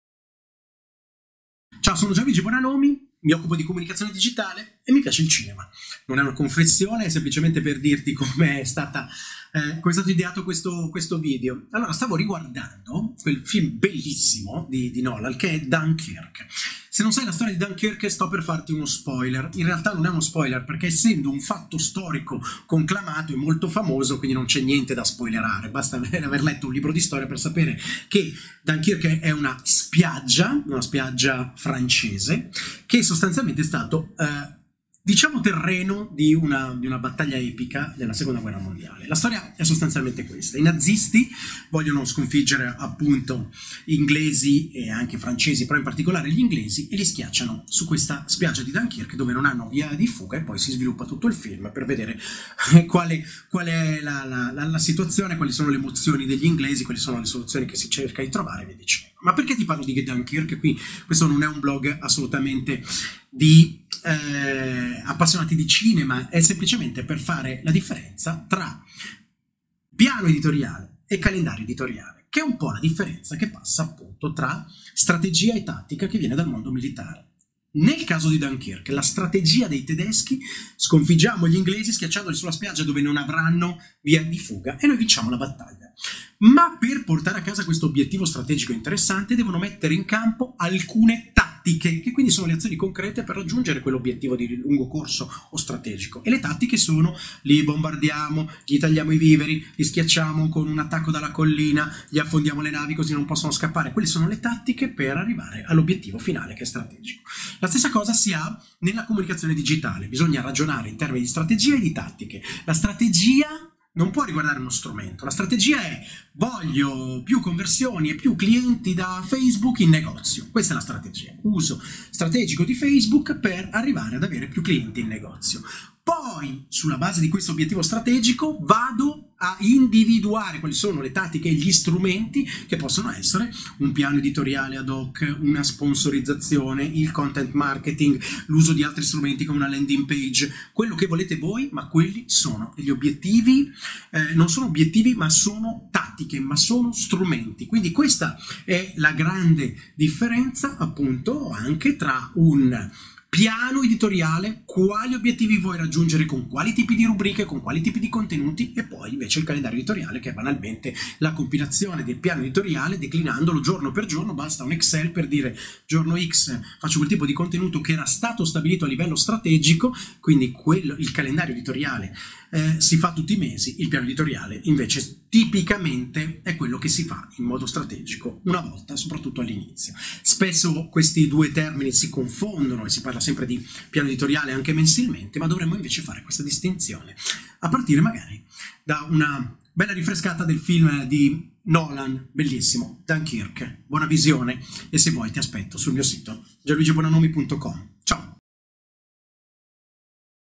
Strategia e tattica: che differenza c'è tra piano e calendario editoriale?
“Un piano editoriale chiaro è imprescindibile per l'efficiente svolgimento dell'attività giornalistica all'interno di qualsiasi testata”. Questa frase non viene da un manuale di giornalismo, bensì da un duro comunicato sindacale di un Comitato di Redazione contro il proprio editore.
Ho fatto il giornalista per quasi vent’anni, e da quel mondo mi porto appresso, anche nel mondo aziendale, alcuni strumenti utili: su tutti, appunto, il piano editoriale. Nel mondo della comunicazione aziendale questo concetto è stato parzialmente stravolto e il piano editoriale indica sia una strategia di comunicazione (mettendo in relazione obiettivi e contenuti), sia un documento programmatico dove inserire, tra le altre cose, delle rubriche. Queste permettono di lavorare con più ordine e soprattutto in anticipo. Il piano editoriale si concretizza poi, di mese in mese, in un calendario editoriale: che cosa pubblicare e quando.
Parto da un bellissimo film (Dunkirk di Nolan) per riprendere i concetti di strategia e tattica e per spiegare, appunto, piano e calendario editoriale.